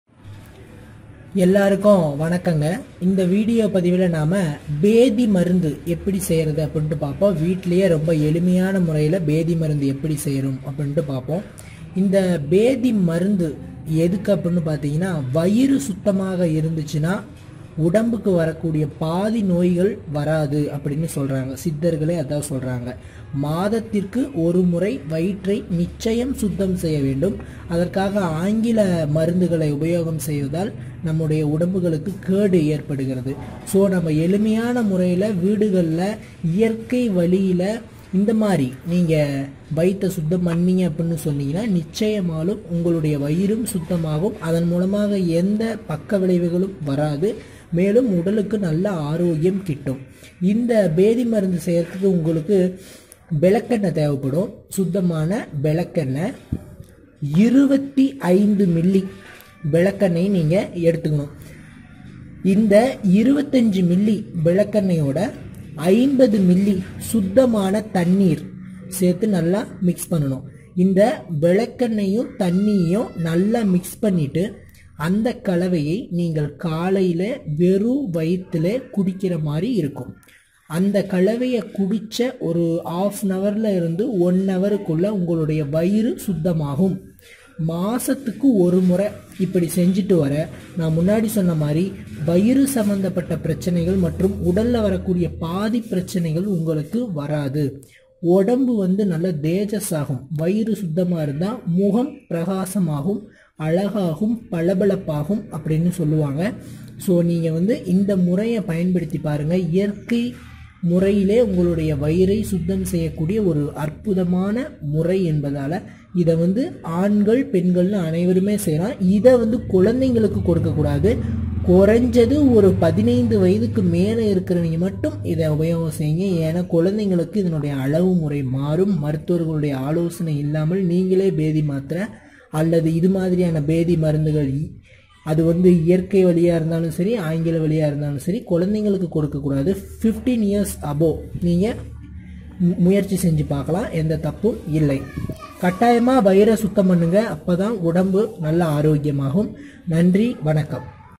ஒரு ரூபாயில் இயற்கையான பேதி மருந்து செய்முறை|666 mantra
இயற்க்கையான முறையில் பேதிமருந்து எப்படி செய்வது என்பதை இந்த பதிவில் காண்போம்.இந்த மருந்தை 15 வயதிற்கு மேற்பட்டவர்கள் உபயோகம் செய்து பயனடையுமாறு கேட்டுக்கொள்கிறேன்.